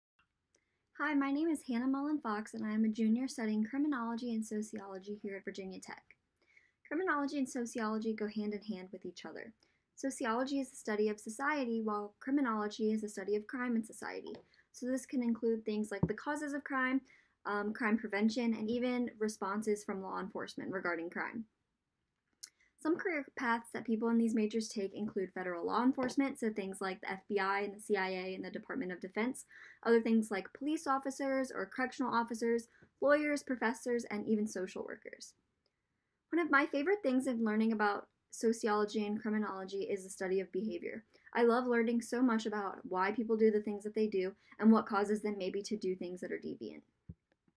Career Corner: Criminology and Sociology
Join the College of Liberal Arts and Human Sciences students as they discuss career opportunities in their majors. Hannah Mullen Fox talks about what you can do with a degree in criminology and sociology in this student-submitted video.